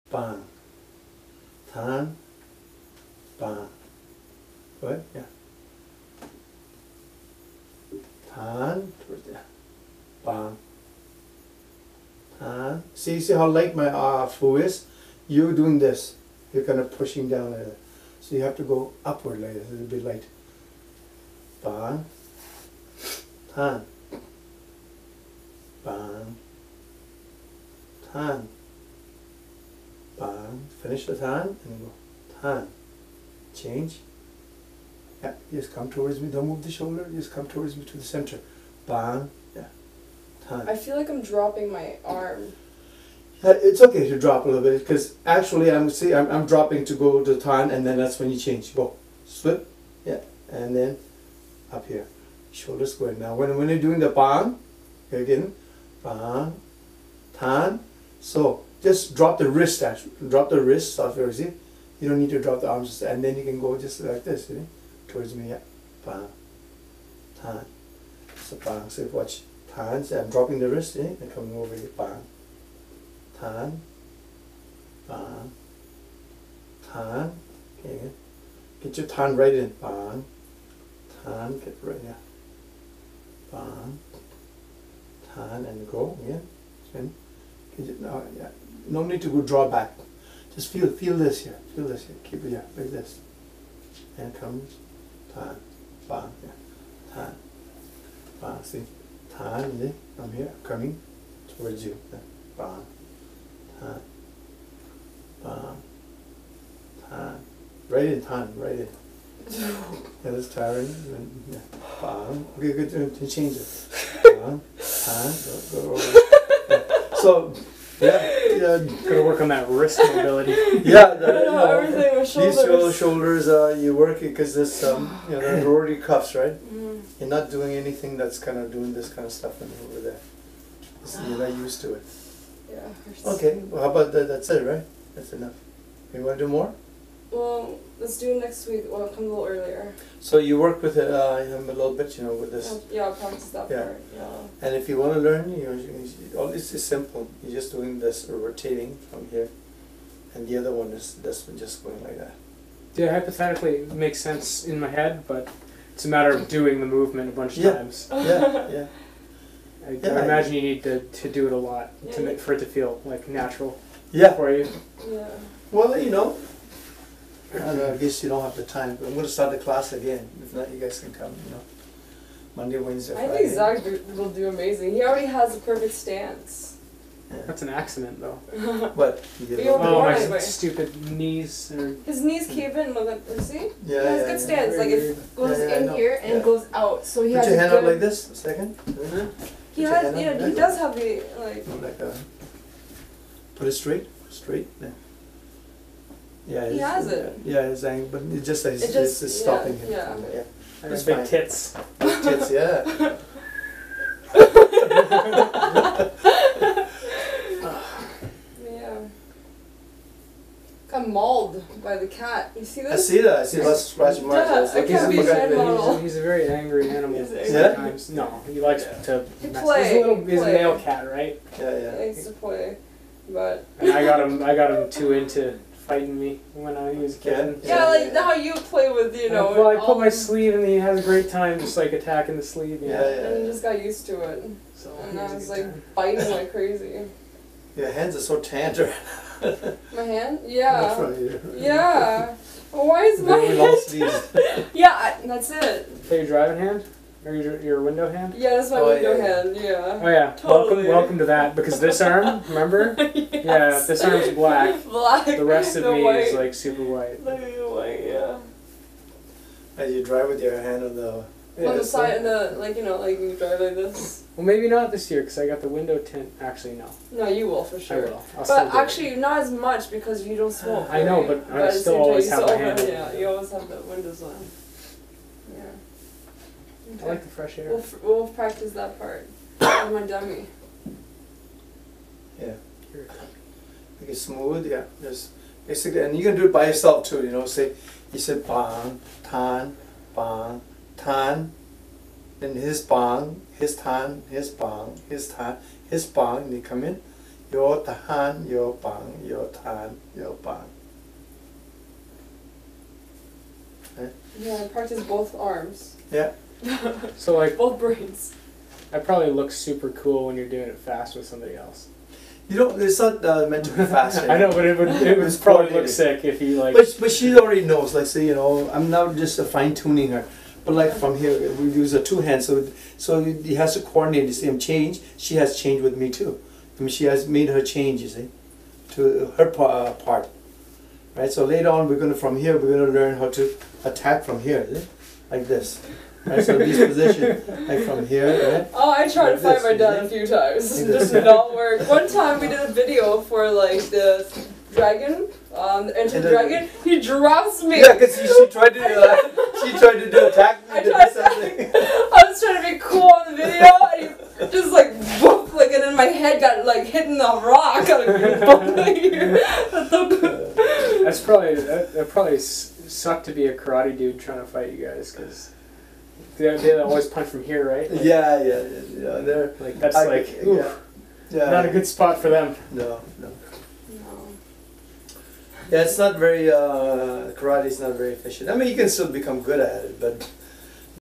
Xuan Wing Chun - Lu Shou Exercise -3
The LuShou (Luksau) exercise should be done slowly to "feel" every moment of your partner's and your own actions.  That's how you train your sensitivity as well as connecting the arm connections to your heels.  It's not to be done speedily without awareness as practiced by many.